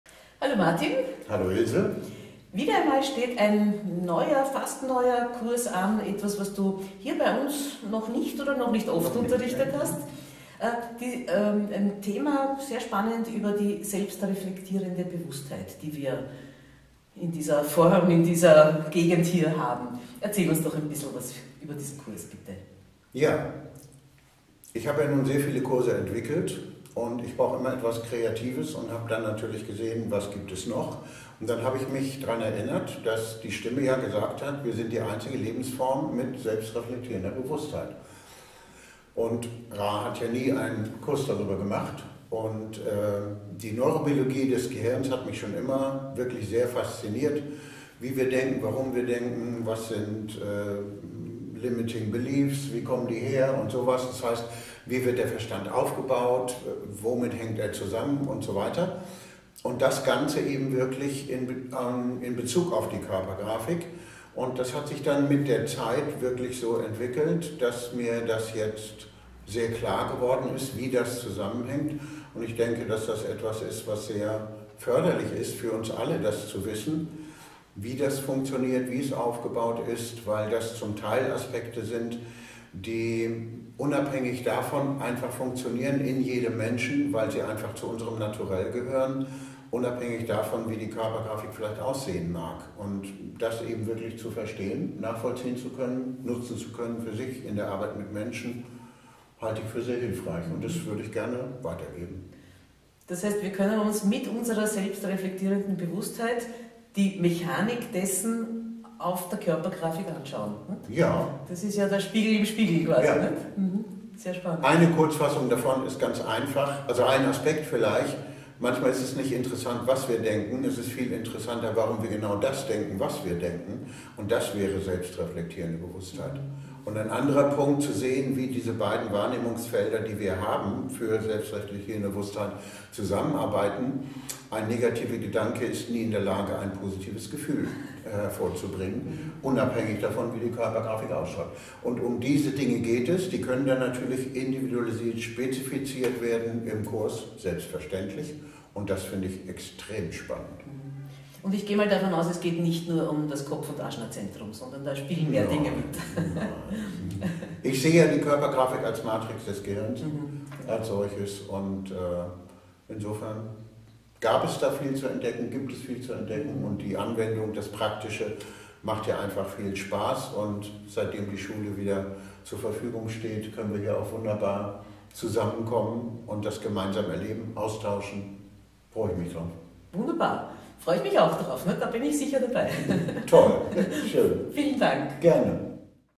Selbstreflektierende Bewusstheit mit Martin Grassinger
Zum ersten Mal LIVE auf deutsch, ein spezielles Human Design Thema vom Gesundheitsspezialisten Martin Grassinger
Tageskurs, LIVE in Wien, 6. Mai 2023